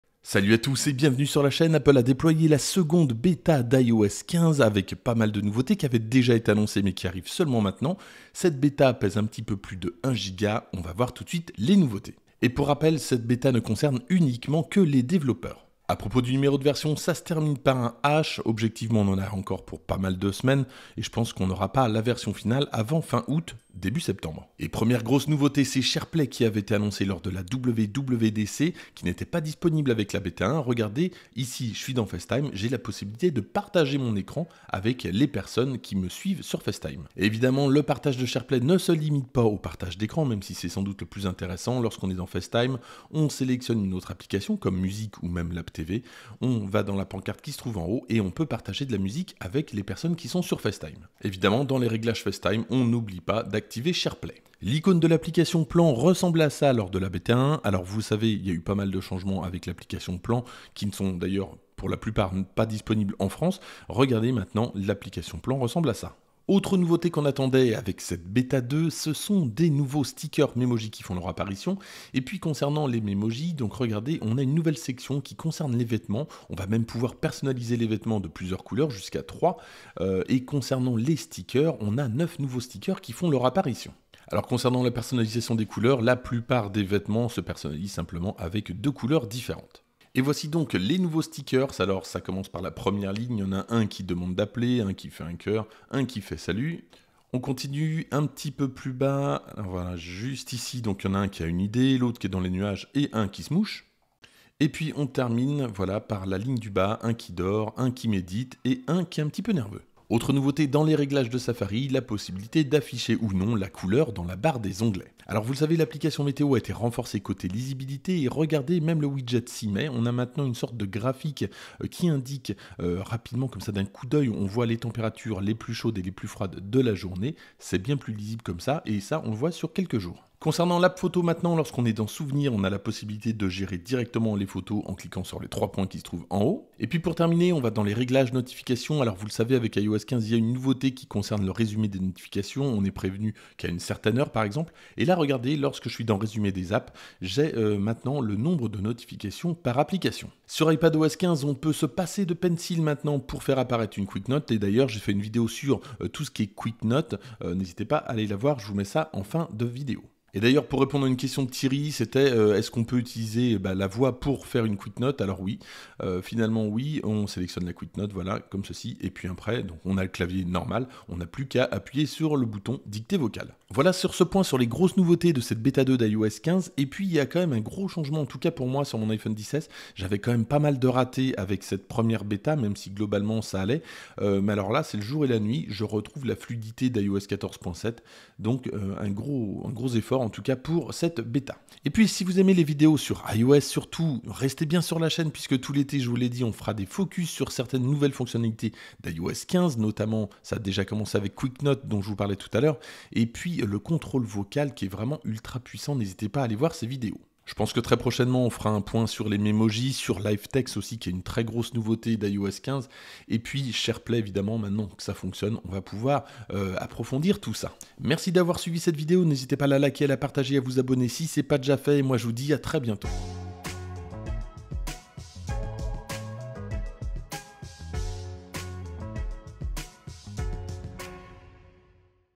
iOS 15 beta 2 : Nouveautés, Memojis, SharePlay et plus encore..
iOS 15 est disponible en beta 2 pour les developpeurs . Je vous propose les nouveautés principales de cette nouvelle version pour iPhone.